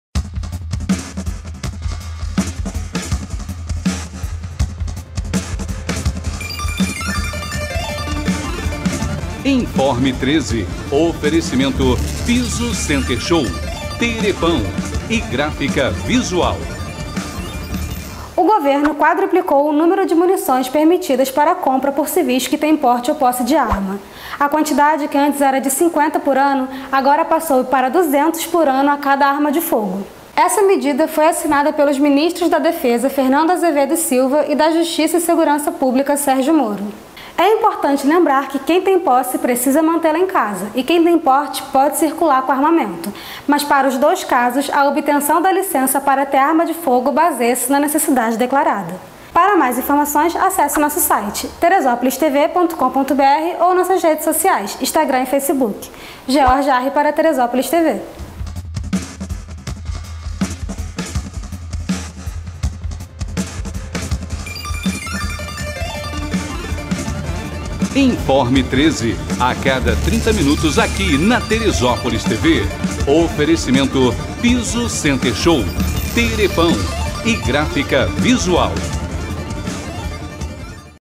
Informe 13 - Número de munições permitidas para civis aumenta - 30.01.2020
Geórgia Harre nos fala sobre a mudança no número de munições permitidas a civis!